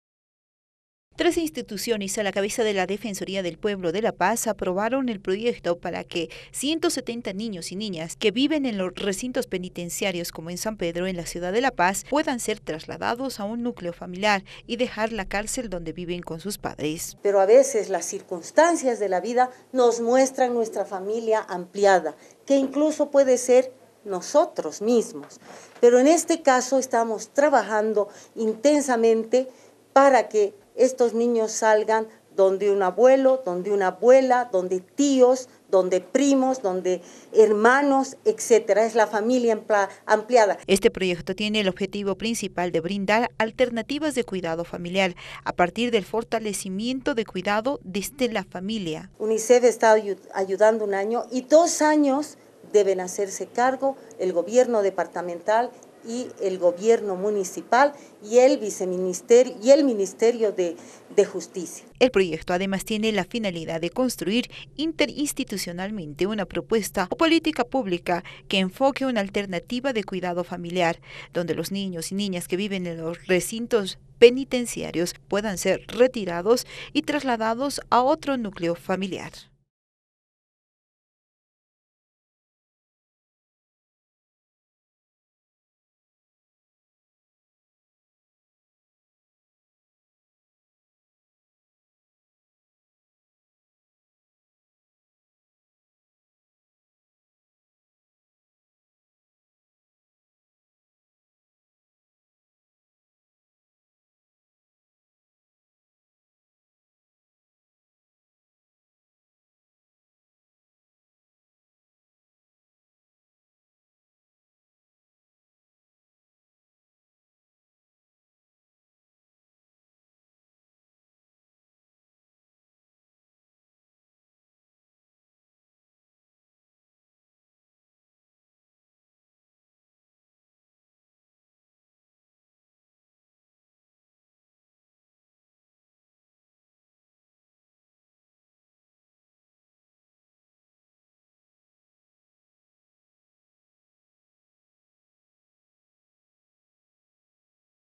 170 niños y niñas que viven en cárceles serán incorporados a un núcleo familiar ampliado
La Paz, Abya Yala Digital.- Tres instituciones, a la cabeza de la Defensoría del Pueblo de La Paz, aprobaron un proyecto para que 170 niños y niñas que viven dentro de recintos penitenciarios puedan ser trasladados a un núcleo familiar y así dejar la cárcel donde viven con sus padres.
La delegada de la Defensoría del Pueblo en La Paz, Teresa Subieta, señaló que este proyecto tiene el objetivo principal de brindar alternativas de cuidado familiar a partir del fortalecimiento del cuidado desde la familia ampliada, como por ejemplo, tíos, primos, hermanos o abuelos.